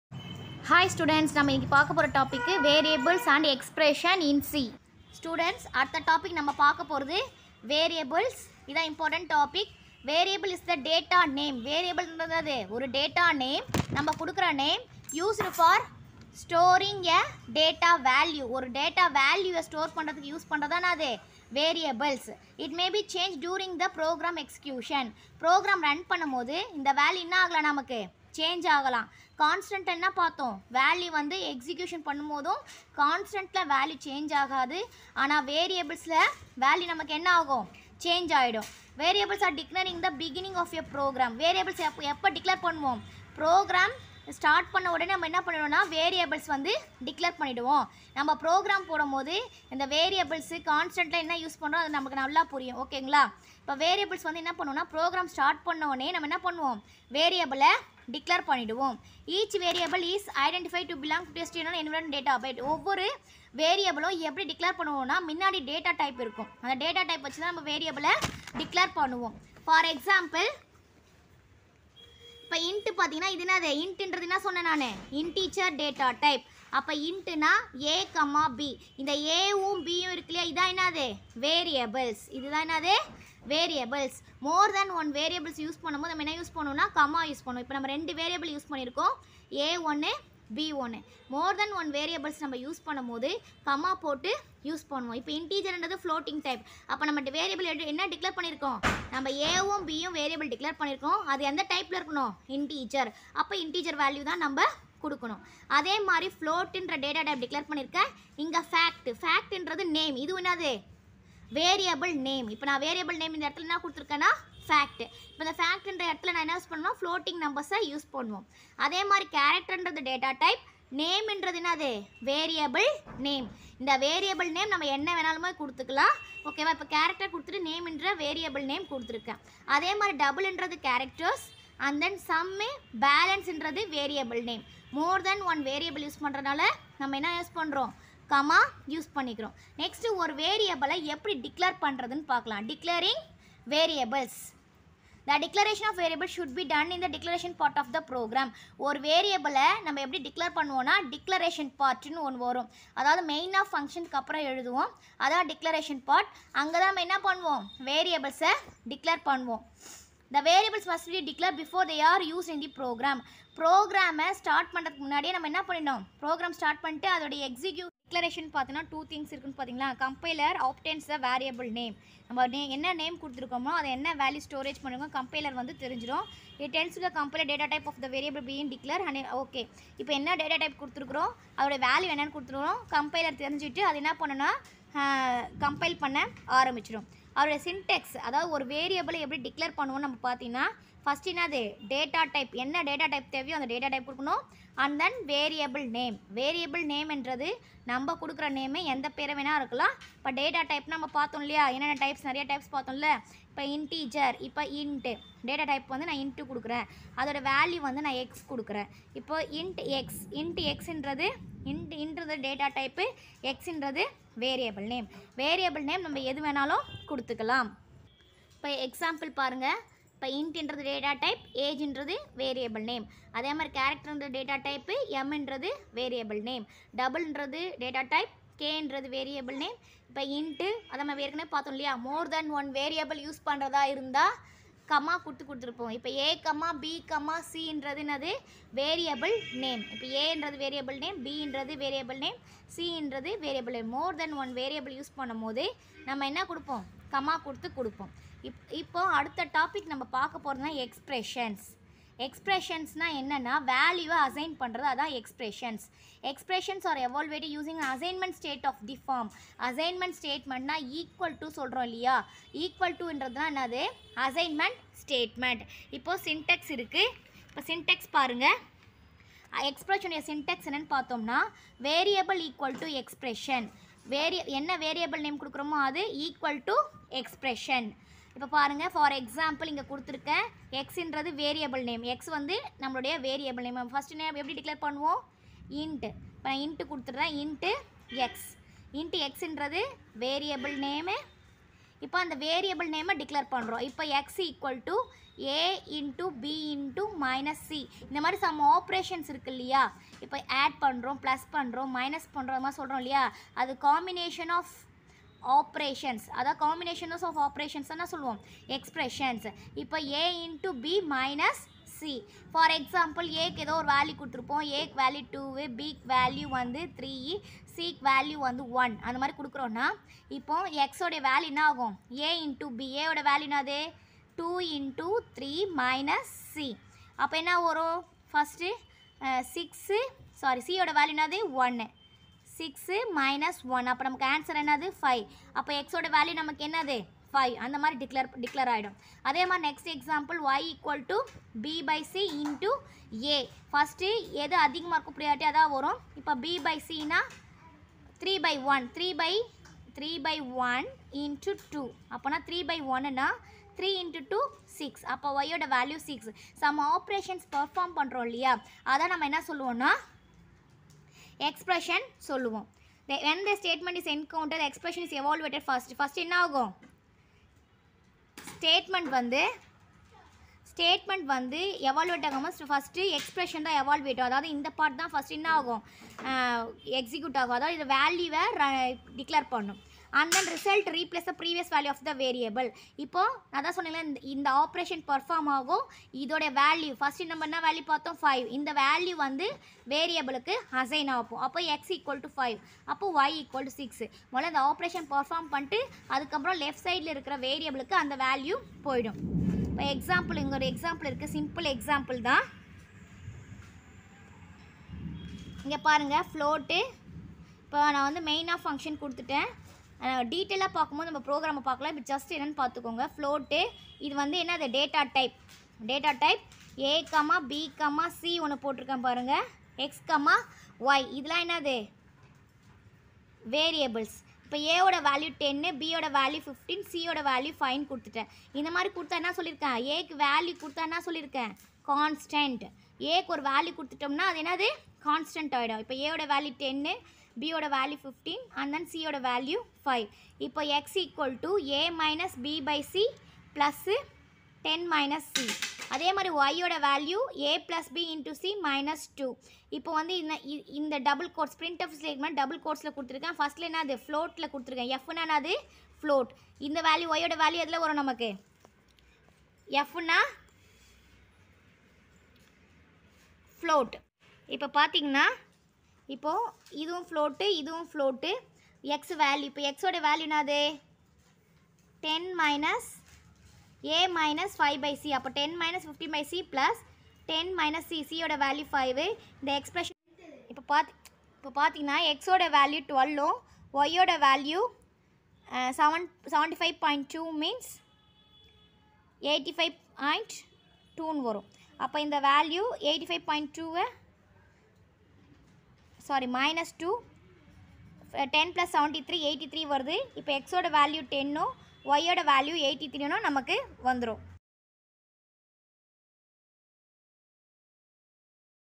Variables and Expression in C | C language in tamil
This video is about variables and expression in c